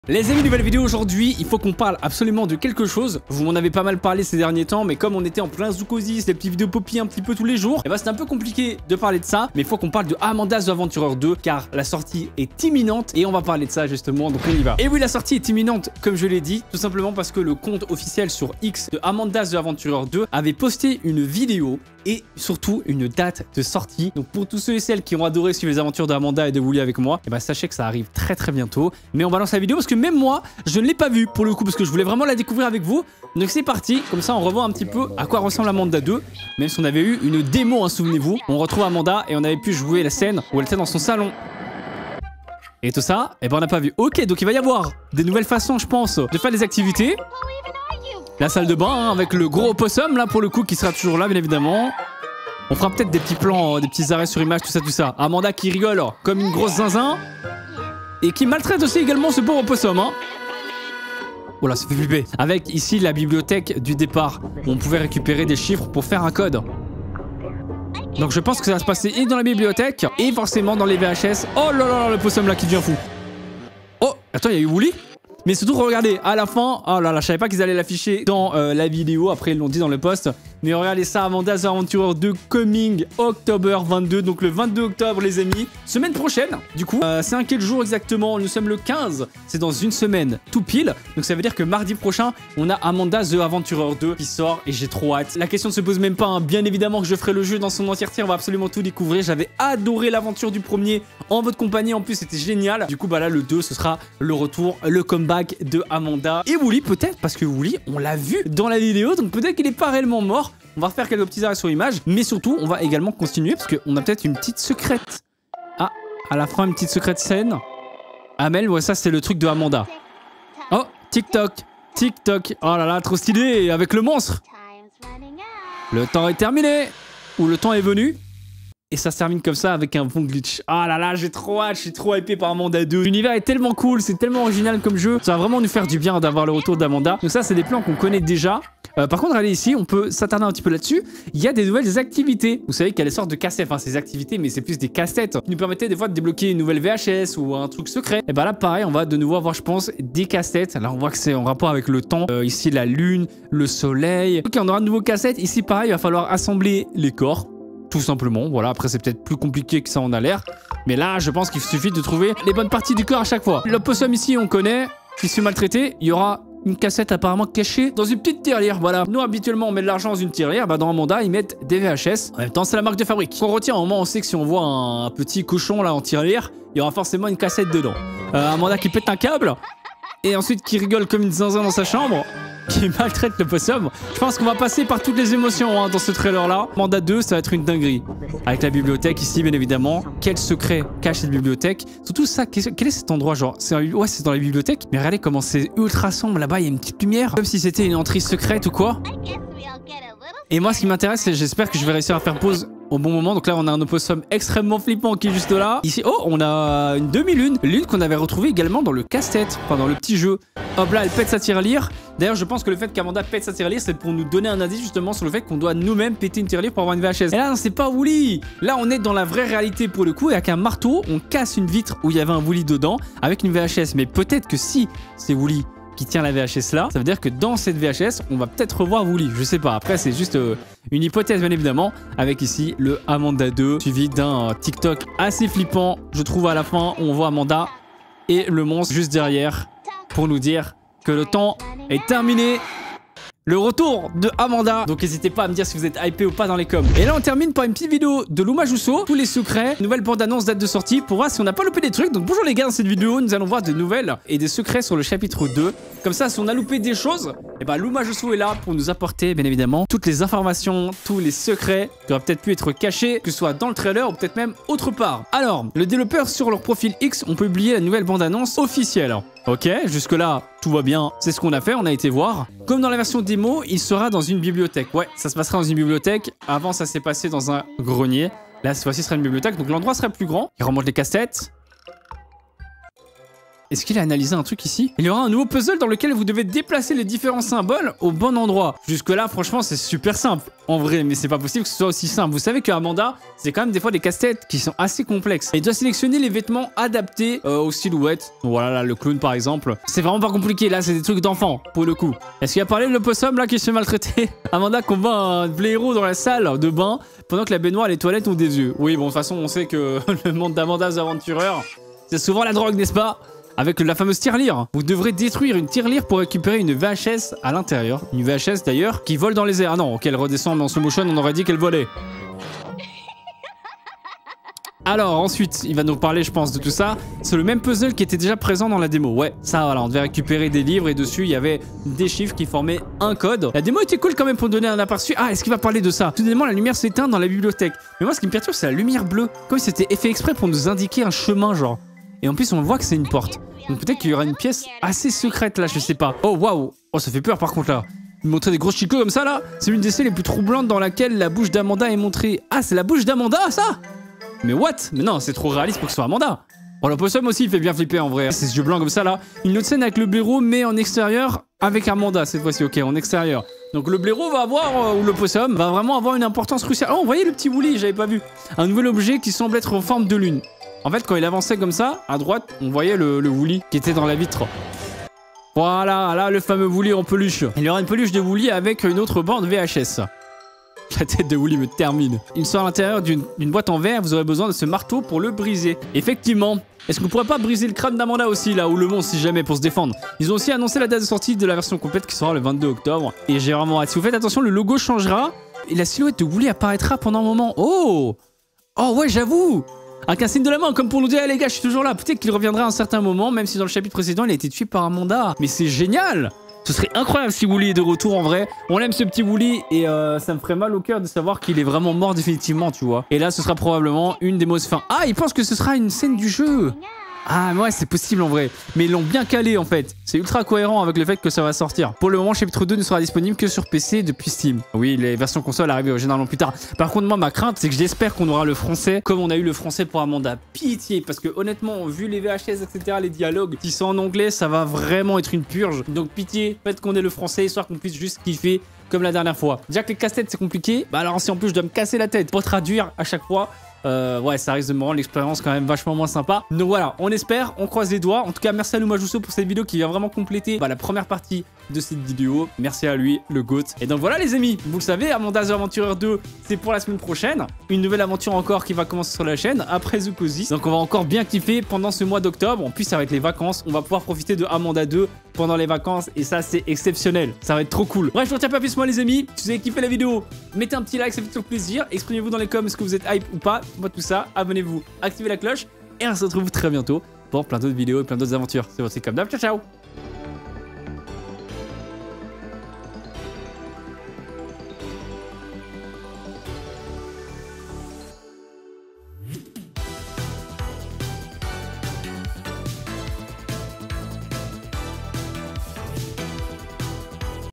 AMANDA THE ADVENTURER 2 : WOOLY DE RETOUR ET DATE DE SORTIE !
Amanda The Adventurer 2 français ( fr ) On décovure la date de sortie !

Si tu veux acheter des objets poppy playtime passe par ce lien